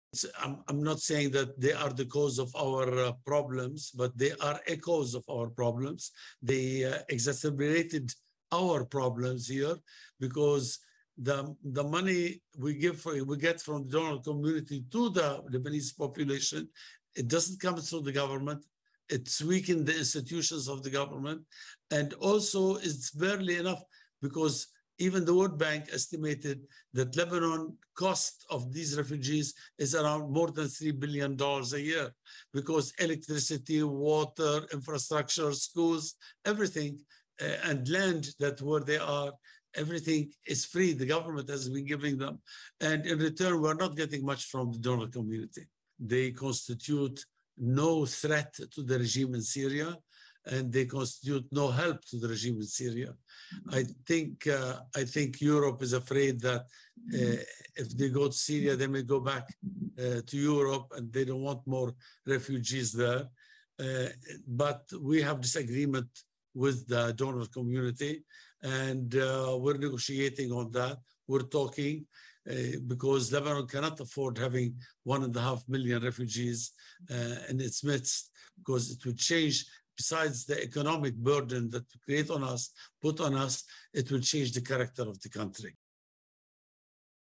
What impact are Syrian refugees having on Lebanon’s crippled economy?
Lebanon hosts about 1.5 million Syrian refugees. The country’s Foreign Minister Abdullah Bou Habib discusses the unique challenges they pose to what the World Bank calls one of the worst economies the world has seen in 100 years.